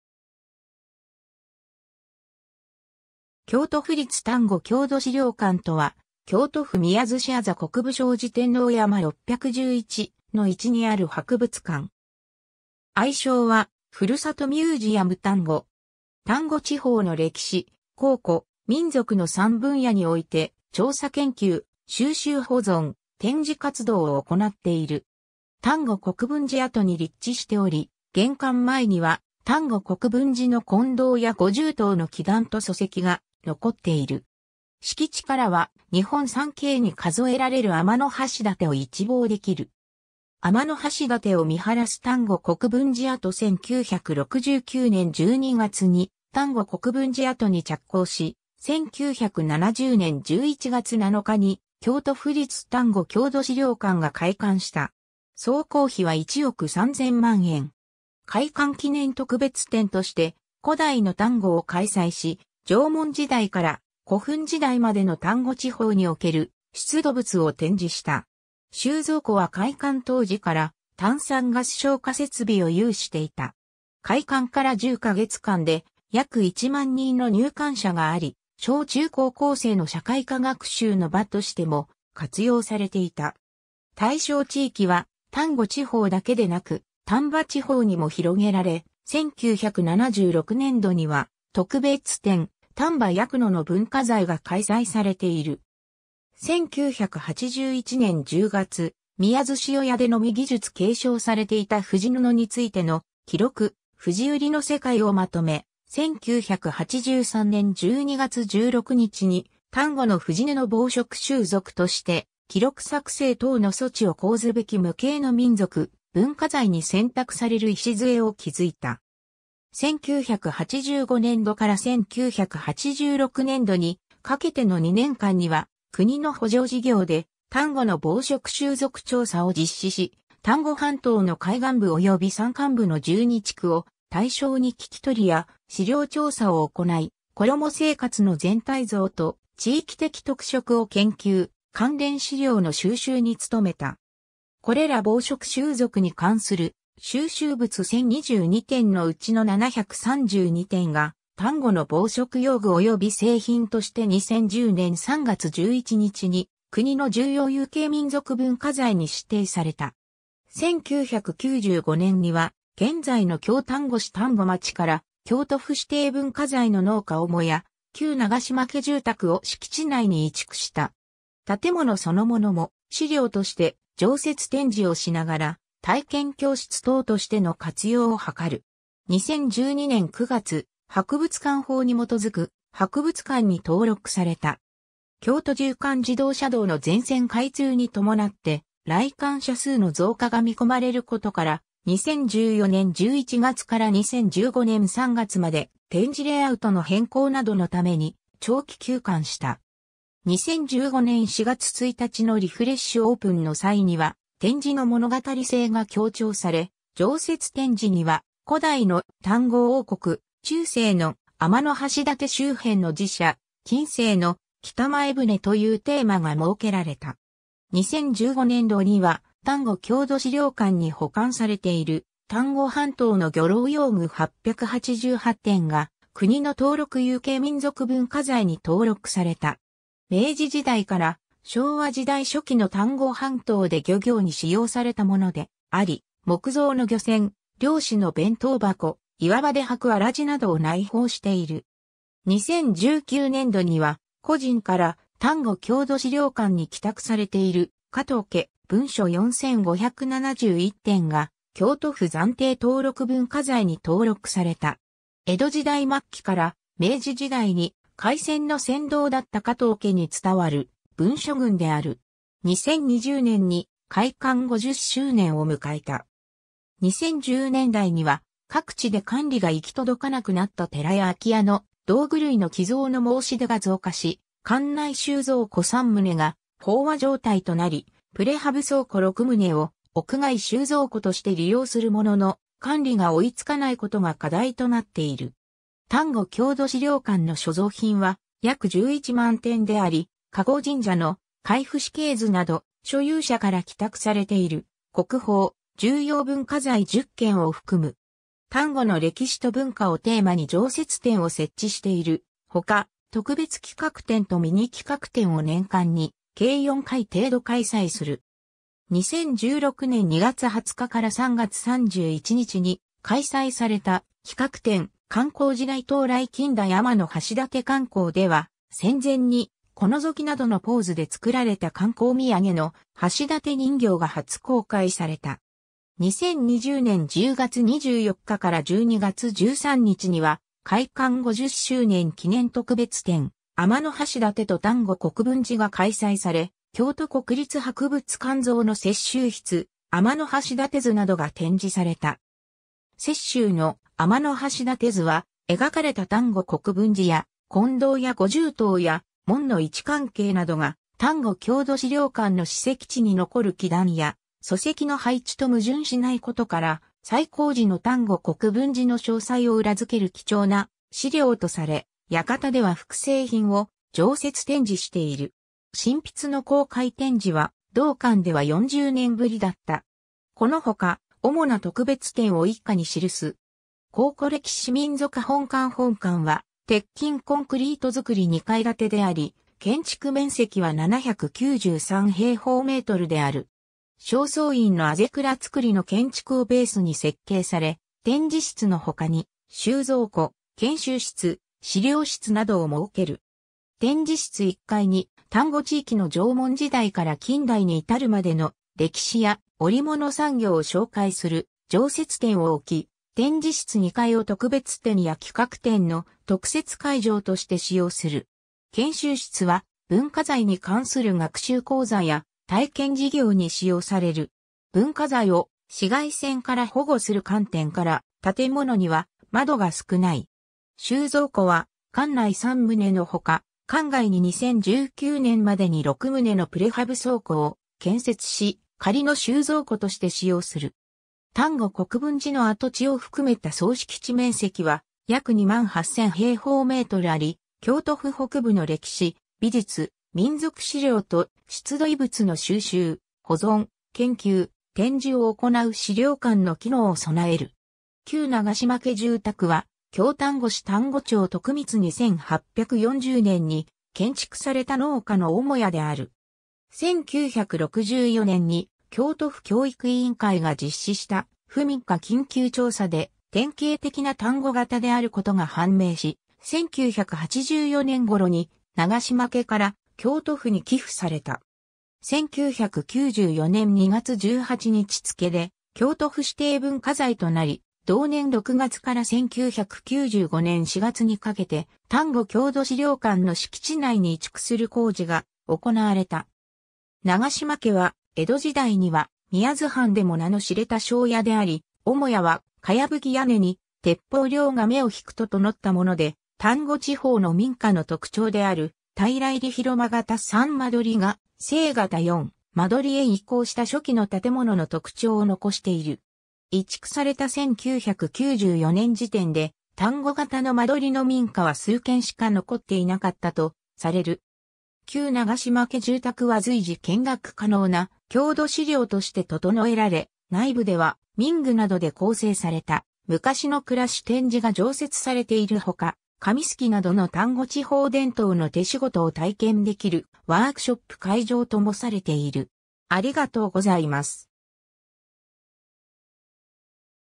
京都府立丹後郷土資料館
京都府立丹後郷土資料館（きょうとふりつたんごきょうどしりょうかん）とは、京都府宮津市字国分小字天王山611-1にある博物館。
愛称はふるさとミュージアム丹後。
丹後地方の歴史・考古・民俗の3分野において、調査研究・収集保存・展示活動を行っている。
 丹後国分寺跡（中世国分寺跡、国の史跡）に立地しており、玄関前には丹後国分寺の金堂や五重塔の基壇と礎石が残っている。
敷地からは日本三景に数えられる天橋立を一望できる。
 天橋立を見晴らす丹後国分寺跡 1969年（昭和44年）12月に丹後国分寺跡に着工し、1970年（昭和45年）11月7日に京都府立丹後郷土資料館が開館した。
総工費は1億3000万円。
開館記念特別展として「古代の丹後」を開催し、縄文時代から古墳時代までの丹後地方における出土物を展示した。
収蔵庫は開館当時から炭酸ガス消火設備を有していた。
 開館から10か月間で約1万人の入館者があり、小中高校生の社会科学習の場としても活用されていた。
対象地域は丹後地方だけでなく丹波地方にも拡げられ、1976年度（昭和51年度）には特別展「丹波夜久野の文化財」が開催されている。
 1981年（昭和56年）10月、宮津市世屋でのみ技術継承されていた藤布についての記録『藤織りの世界』をまとめ、1983年（昭和58年）12月16日に「丹後の藤布紡織習俗」として記録作成等の措置を講ずべき無形の民俗文化財に選択される礎を築いた。
 1985年度（昭和60年度）から1986年度（昭和61年度）にかけての2年間には、国の補助事業で「丹後の紡織習俗調査」を実施し、丹後半島の海岸部及び山間部の12地区を対象に聞き取りや資料調査を行い、衣生活の全体像と地域的特色を研究、関連資料の収集に努めた。
これら紡織習俗に関する収集物1,022点のうちの732点が、「丹後の紡織用具及び製品」として2010年（平成22年）3月11日に国の重要有形民俗文化財に指定された。
 1995年（平成7年）には、現在の京丹後市丹後町から京都府指定文化財の農家母屋「旧永島家住宅」を敷地内に移築した。
建物そのものも資料として常設展示をしながら、体験教室等としての活用を図る。
 2012年（平成24年）9月、博物館法に基づく博物館に登録された（登録博物館）。
 京都縦貫自動車道の全線開通にともなって来館者数の増加が見込まれることから、2014年（平成26年）11月から2015年（平成27年）3月まで、展示レイアウトの変更などのために長期休館した。
2015年（平成27年）4月1日のリフレッシュオープンの際には展示の物語性が強調され、常設展示には古代の「丹後王国」、中世の「天橋立周辺の寺社」、近世の「北前船」というテーマが設けられた。
 2015年度（平成27年度）には、丹後郷土資料館に保管されている「丹後半島の漁撈用具」888点が国の登録有形民俗文化財に登録された。
明治時代から昭和時代初期の丹後半島で漁業に使用されたものであり、木造の漁船、漁師の弁当箱、岩場で履くわらじなどを内包している。
 2019年度（令和元年度）には、個人から丹後郷土資料館に寄託されている「加藤家文書」4571点が京都府暫定登録文化財に登録された。
江戸時代末期から明治時代に廻船の船頭だった加藤家に伝わる文書群である。
 2020年（令和2年）に開館50周年を迎えた。
2010年代には、各地で管理が行き届かなくなった寺や空き家の道具類の寄贈の申し出が増加し、館内収蔵庫3棟が飽和状態となり、プレハブ倉庫6棟を屋外収蔵庫として利用するものの、管理が追いつかないことが課題となっている。
 丹後郷土資料館の所蔵品は約11万点であり、籠神社の「海部氏系図」（国宝）など、所有者から寄託されている国宝・重要文化財10件を含む。
丹後の歴史と文化をテーマに常設展を設置しているほか、特別企画展とミニ企画展を年間に計4回程度開催する。
 2016年（平成28年）2月20日から3月31日に開催された企画展「観光時代到来 近代天橋立観光」では、戦前に股のぞきなどのポーズで作られた観光土産の橋立人形が初公開された。
 2020年（令和2年）10月24日から12月13日には、開館50周年記念特別展「天橋立と丹後国分寺」が開催され、京都国立博物館蔵の雪舟筆「天橋立図」（国宝）などが展示された。
雪舟の「天橋立図」は、描かれた丹後国分寺や金堂や五重塔や門の...